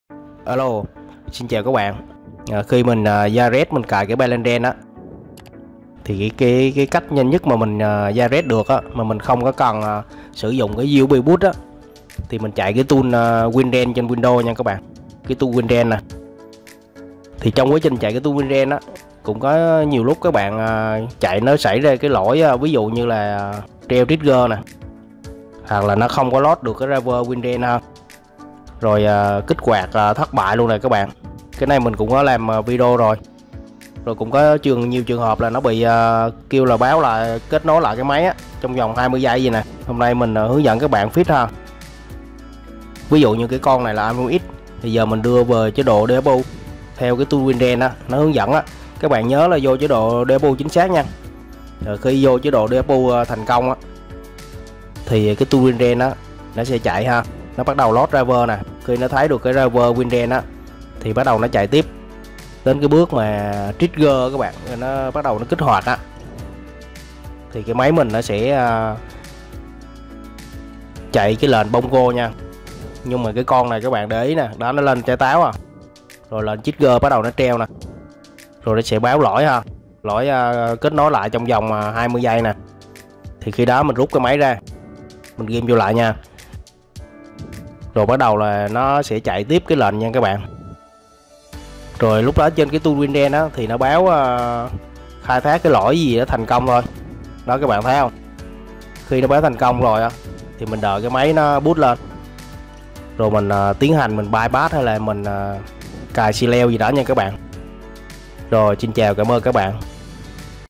[WinRa1n] Fix Bugs [STAGE]: Please reconnect your device within 20s...(Failed)
Cách sửa lỗi kết nối lại khi Jailbreak bằng WinRa1n trên Windows | Bypass iOS 15, 16 cho iPhone, iPad | không cần USB | cài đặt Palera1n | By CheckRa1n 0.13

"ting ting" ủng hộ cốc trà đá nha các bạn🤩.. ACB: 6962377 (MOMO: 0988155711)